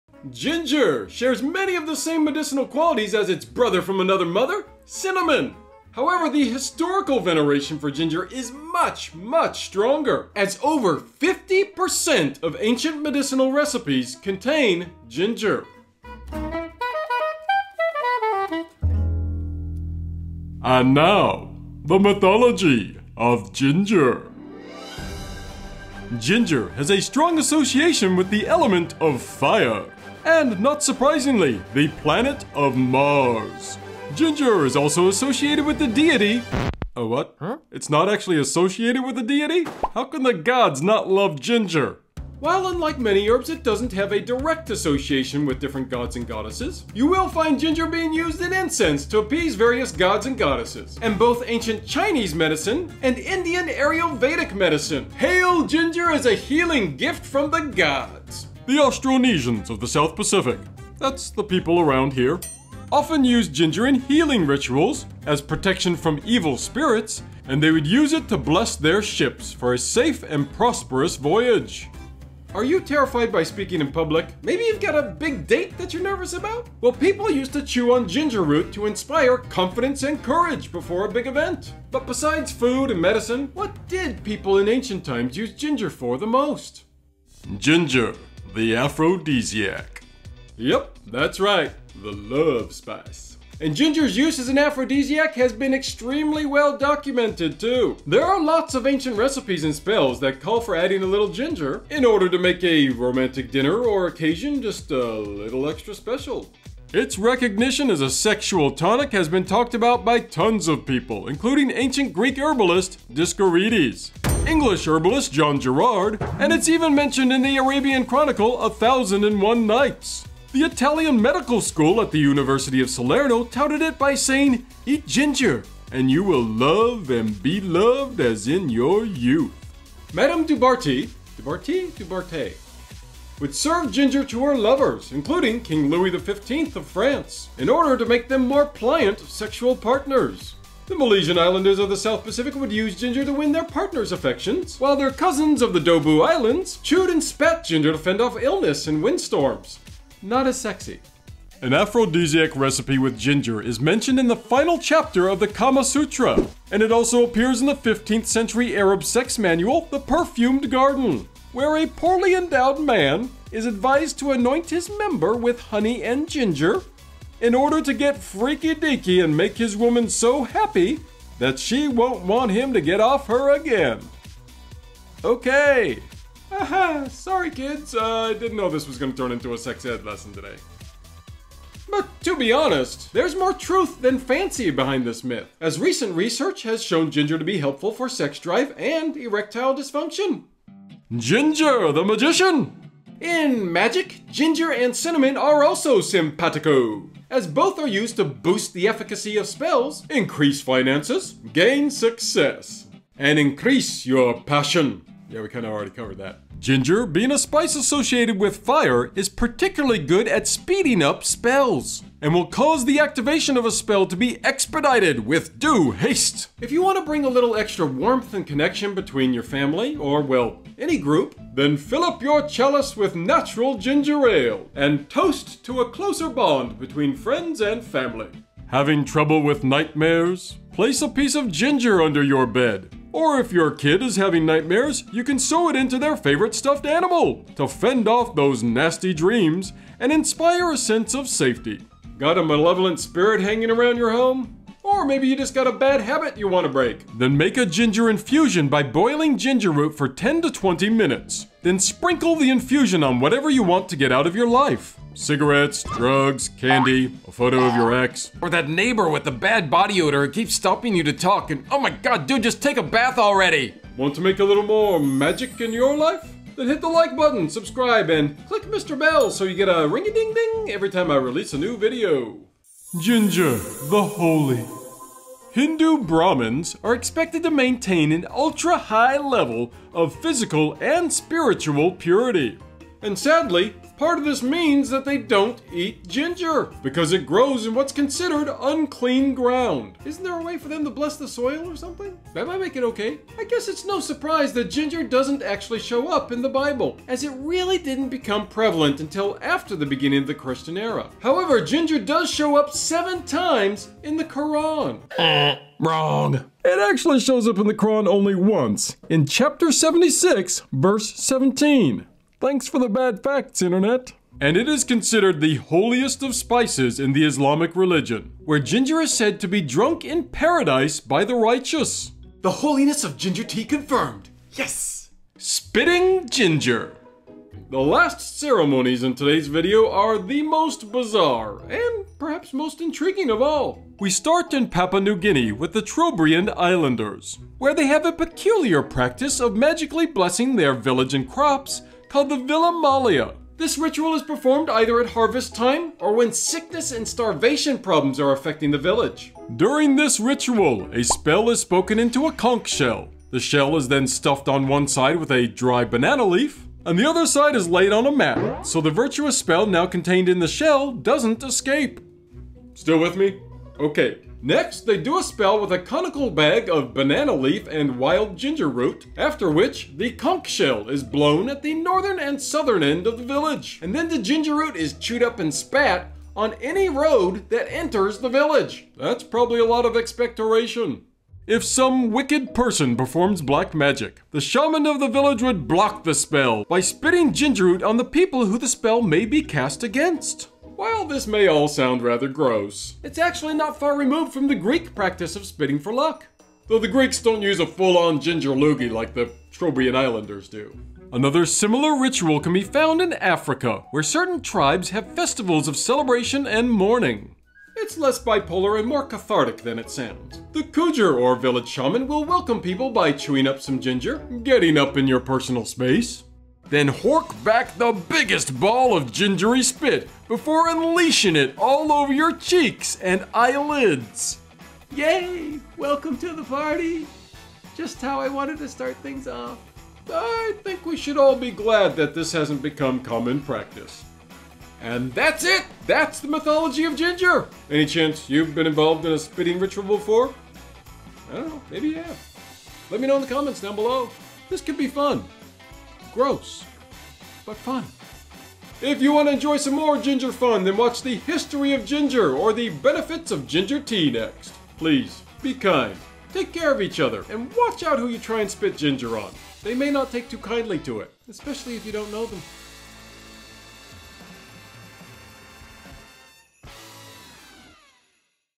The MYTHOLOGY of GINGER | Ginger folklore, magic & religious beliefs
Ginger has tons of crazy things that people have believed about it over the ages. Even spitting on each other. What? Yup. Find out by watching this video!

Timestamps-
0:00 Intro
0:28 Gods & Goddesses
1:26 Ginger the aphrodisiac
3:11 Ginger the Magician
4:31 Ginger the Holy
5:25 Spitting Ginger